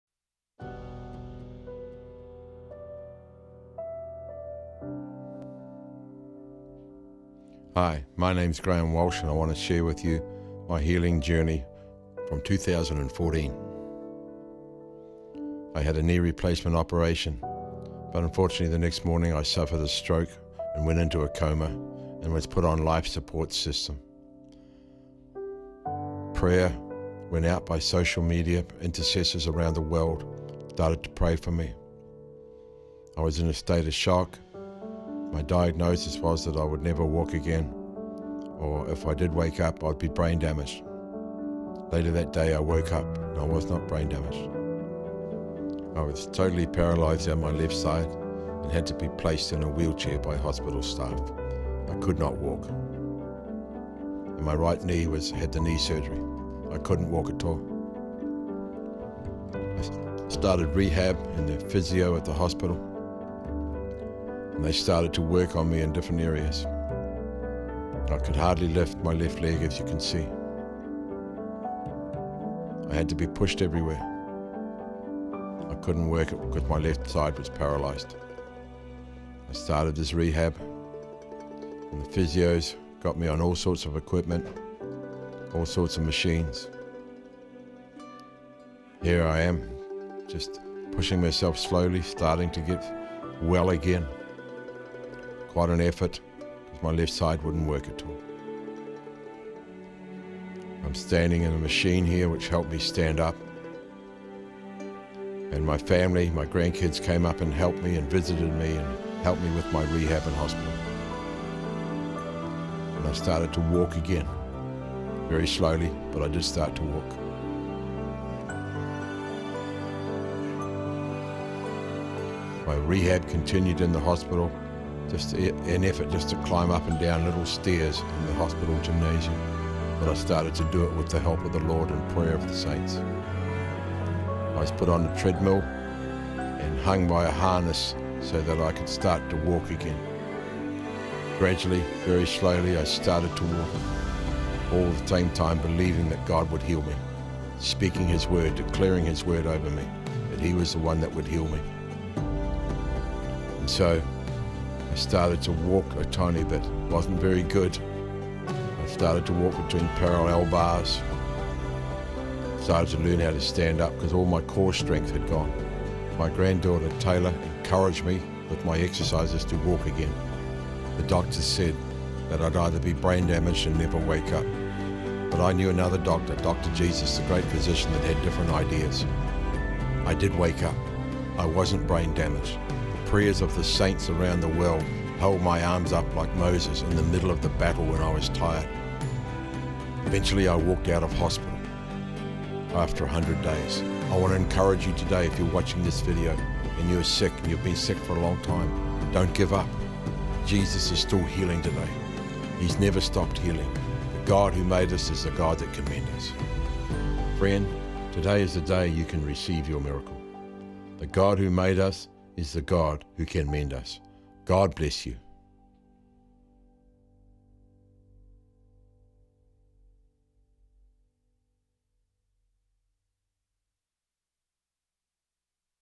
Our Journey of Healing
We have a miracle working God, against all medical opinions Graeme is well on his way to recovery.
 The God who made us is the God who heals us.